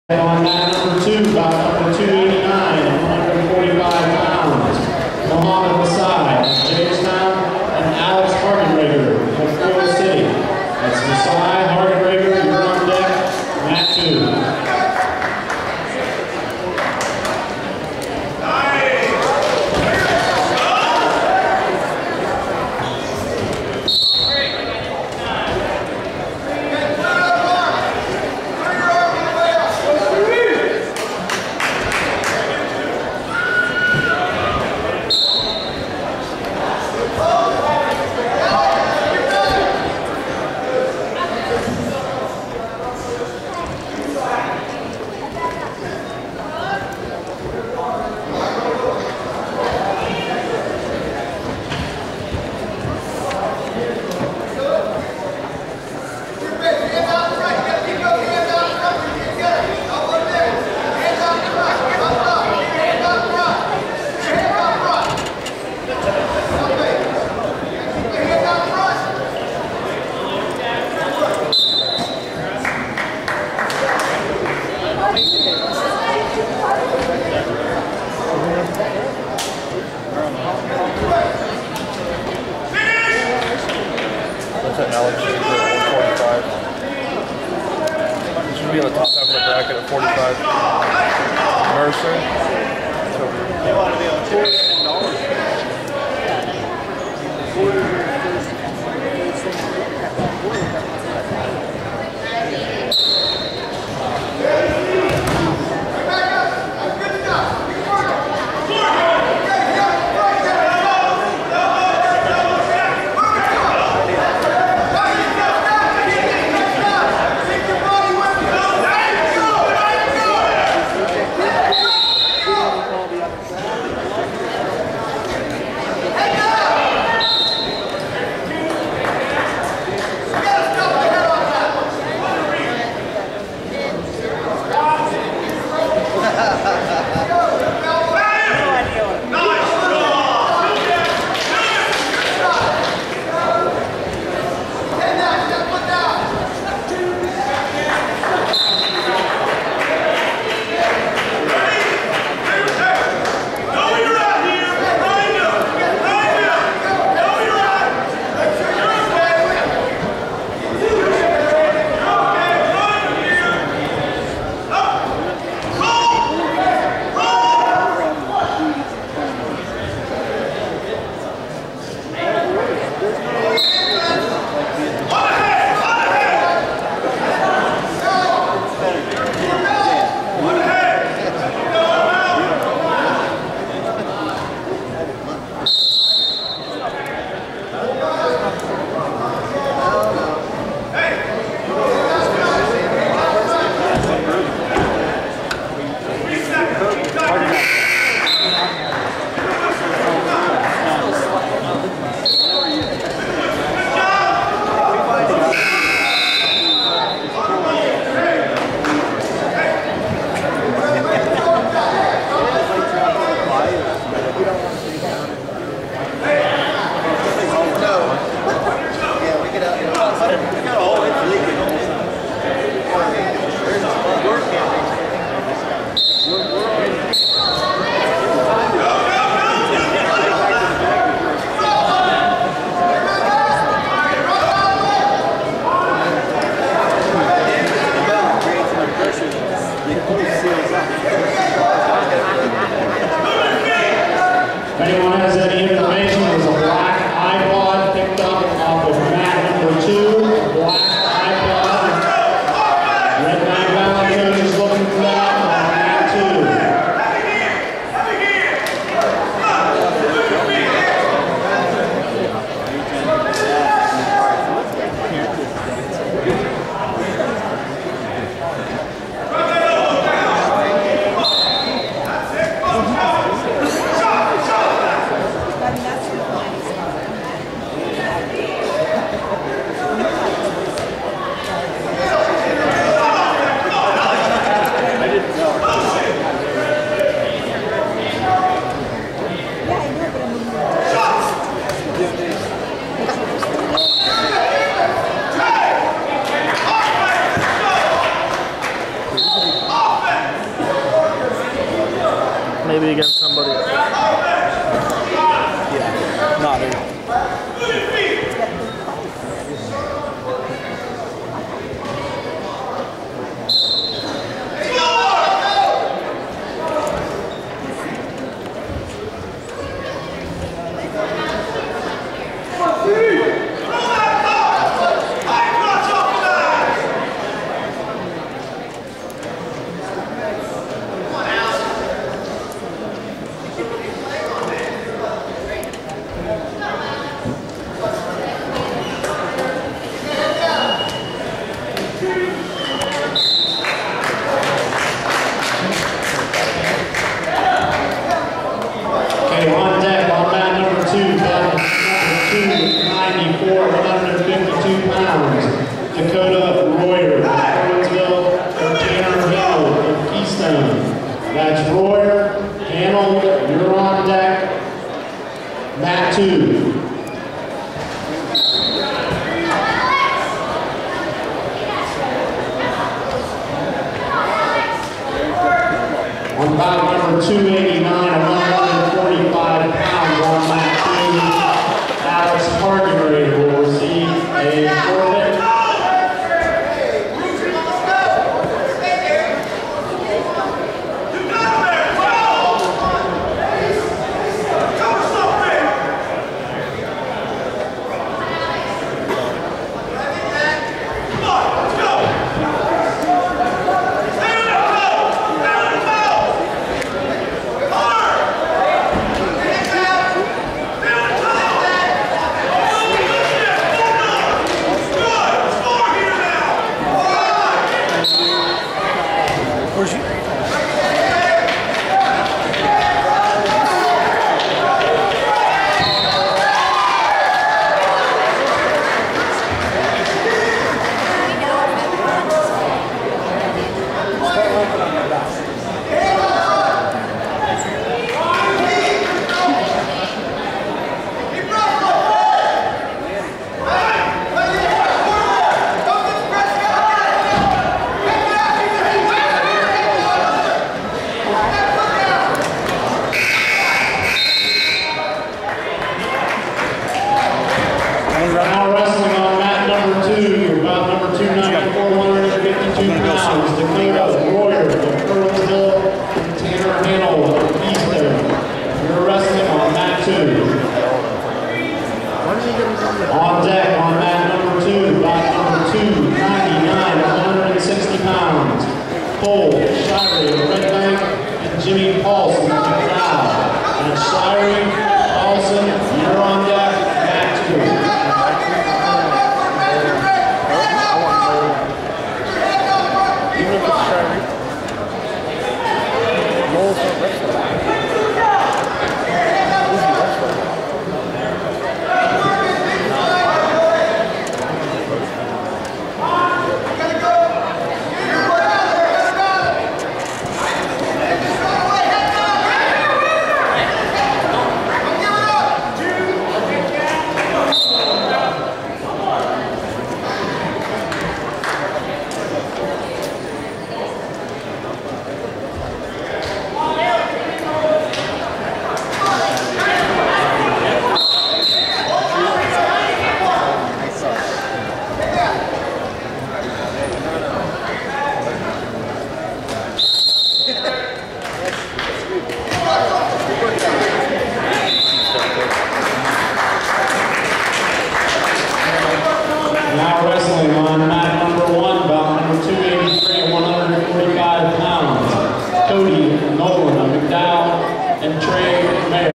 Steve Spearman (McDowell) vs. Taylor Ellis (Conneaut Lake)
2012 Fred Bell Tournament: In the 138 pound quarter finals Steve Spearman (McDowell) won by tech fall 22-7 at 5:35 over Taylor Ellis (Conneaut Lake)